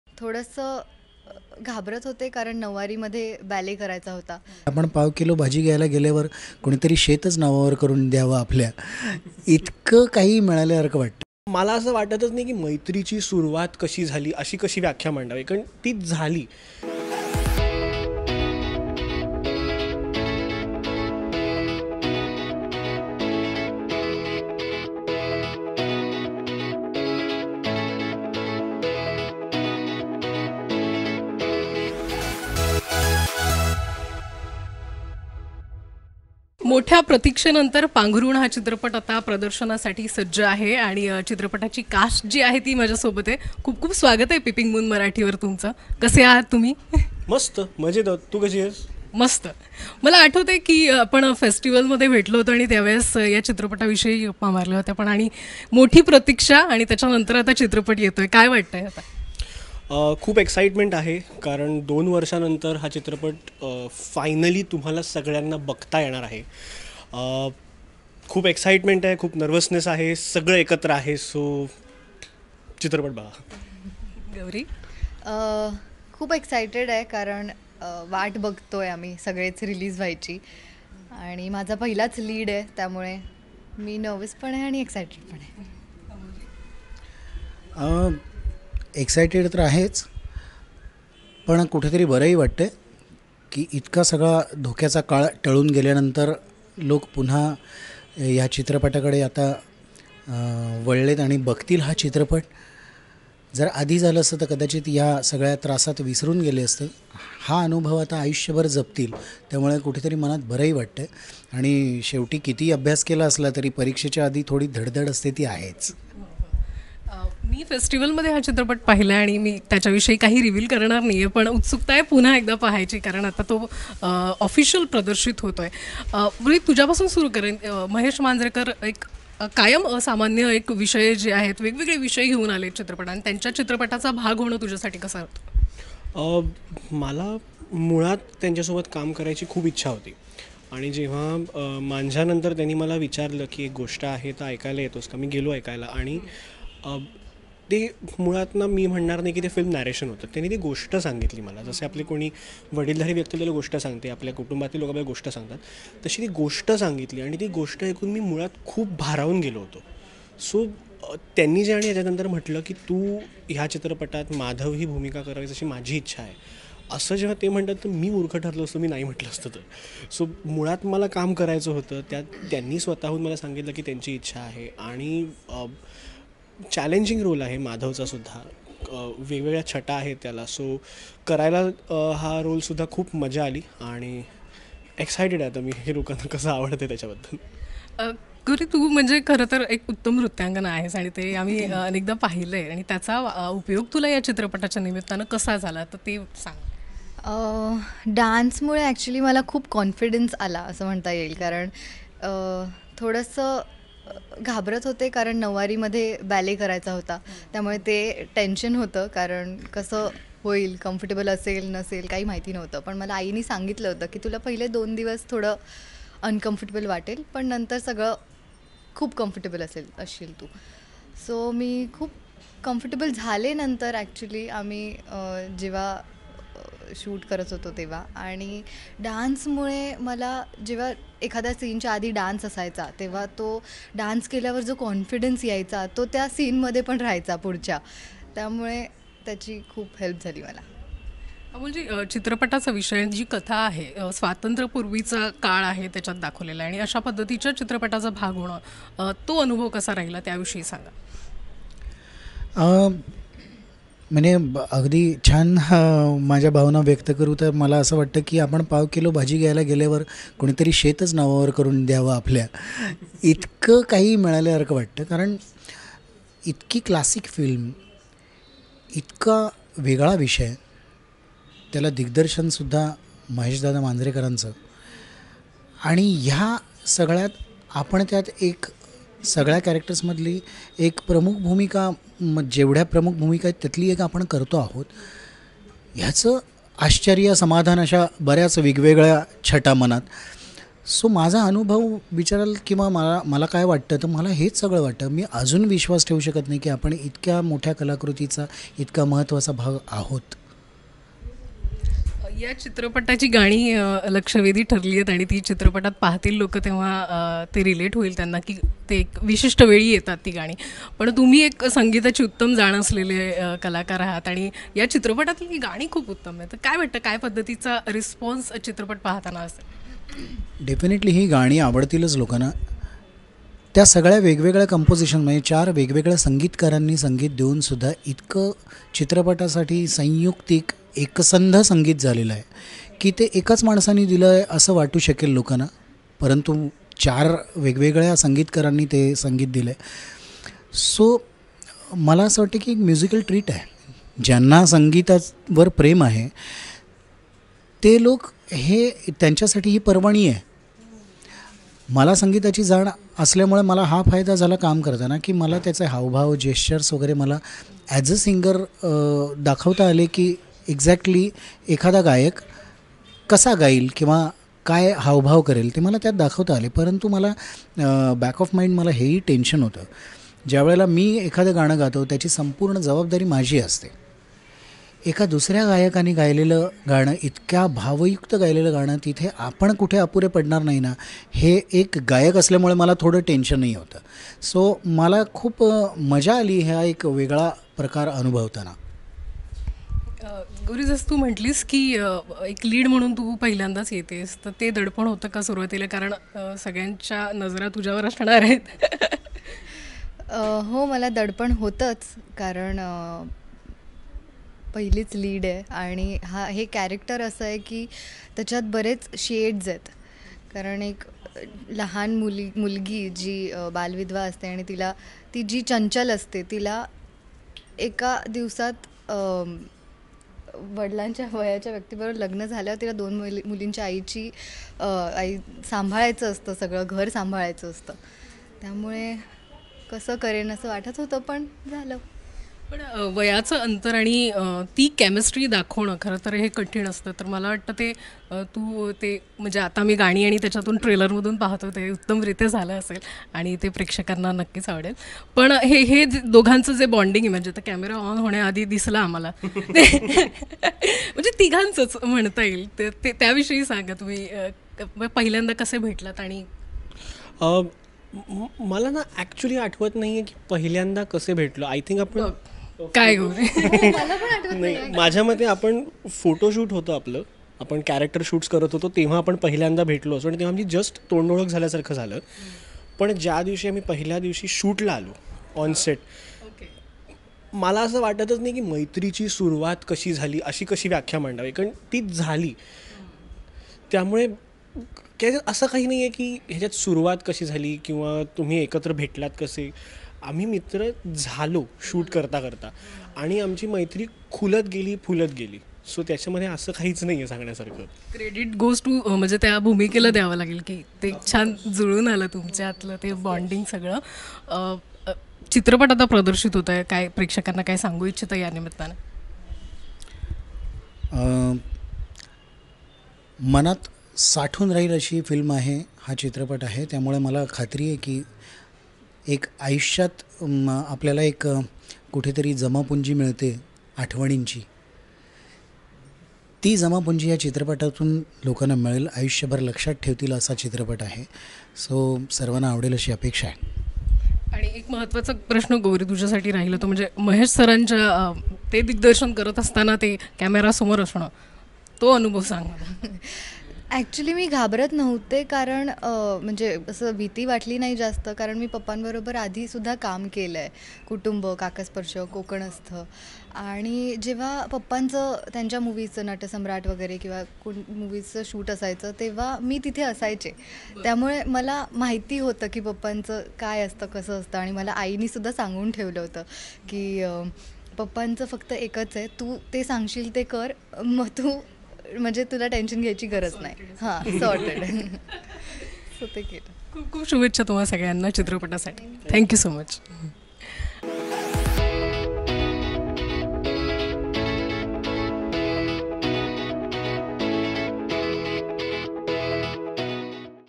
In coversation with the cast of 'Panghrun'
In an exclusive chat with PeepingMoon Marathi Gauri Ingawale, Amol Bawdekar and Rohit Phalke get candid about their marathi movie 'Panghrun'.

For the latest on Bollywood, Fashion & TV updates do check out